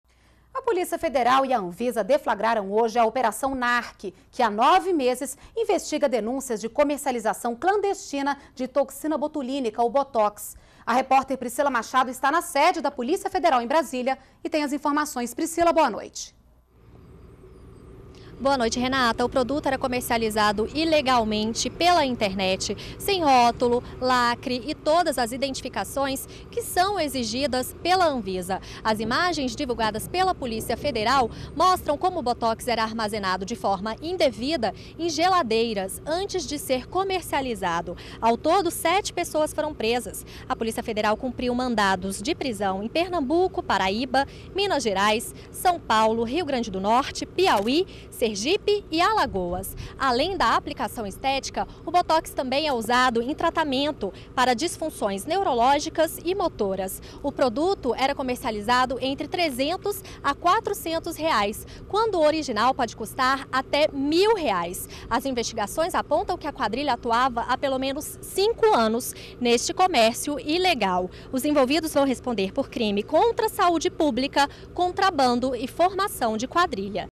Deflagrada operação que investigava comercialização clandestina de botox
NBR NOTÍCIAS - 03.04.12: A Polícia Federal e a Anvisa, deflagraram nesta terça-feira (3) a "Operação Narke" que há 9 meses investiga denúncias de comercialização clandestina de toxina botulínica, o botox.